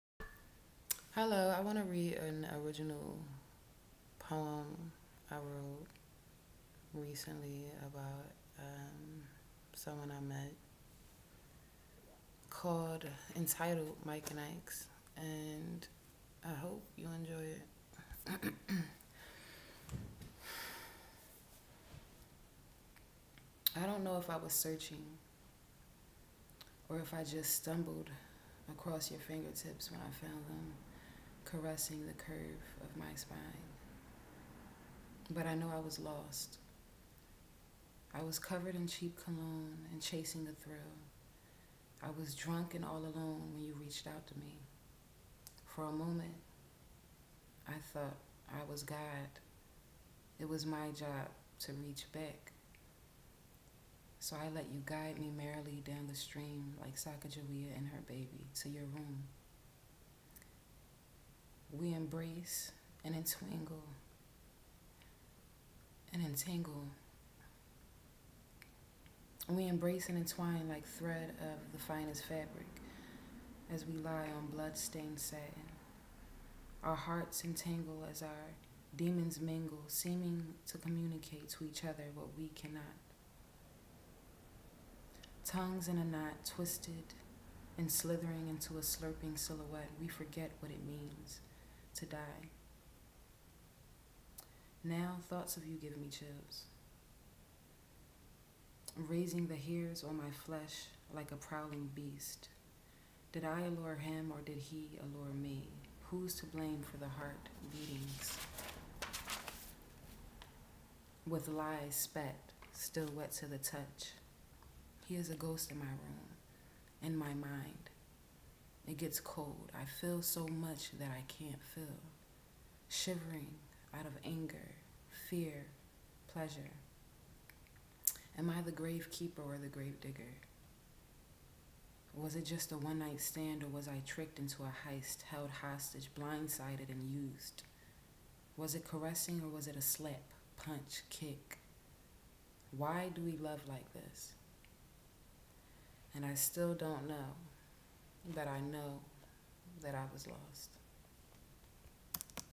(Spoken Word/Poetry) Mike & Ike's - Kimberly Rushing
Original poem I wrote about misogyny, among other oppressive and abusive mental illness and the human tragic flaw. enjoy:)